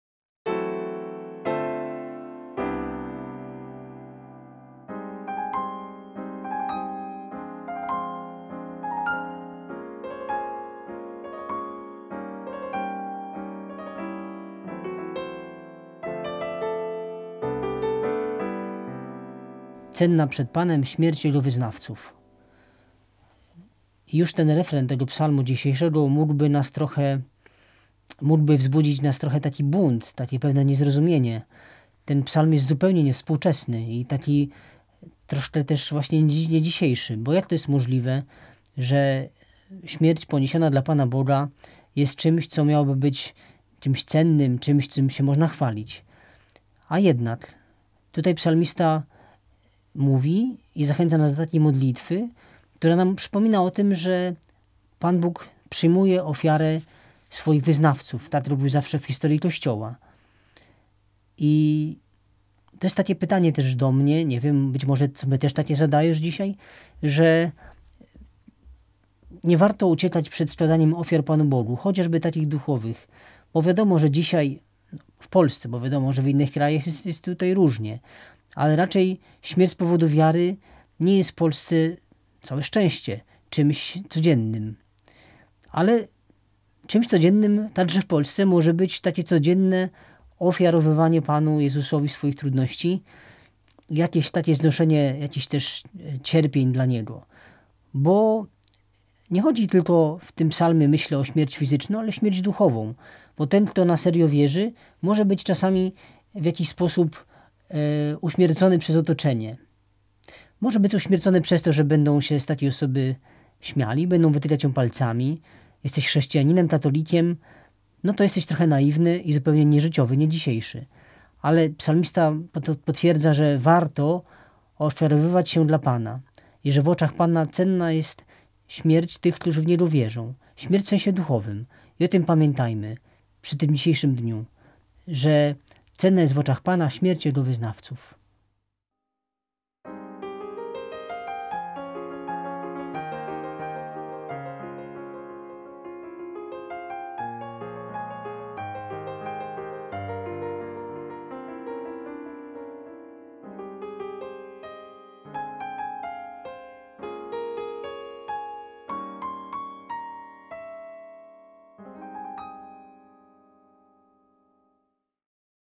SłowoOPsalmach | 25 lipca 2018 | ks. Piotr Niedzielski
Komentarz do psalmu responsoryjnego
Ps 116B, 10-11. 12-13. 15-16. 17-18

     Cenna przed Panem śmierć Jego wyznawców.

Ufność miałem nawet wtedy, gdy mówiłem:
„Jestem w wielkim ucisku”.
I zalękniony wołałem:
„Każdy człowiek jest kłamcą”.

Czym się Bogu odpłacę
za wszystko, co mi wyświadczył?
Podniosę kielich zbawienia
i wezwę imienia Pana.

Cenna jest w oczach Pana
śmierć jego wyznawców.
Panie, jestem Twoim sługą,
Twym sługą, synem Twojej służebnicy,
Ty rozerwałeś moje kajdany.

Tobie złożę ofiarę pochwalną
Wypełnię me śluby dla Pana
przed Jego ludem.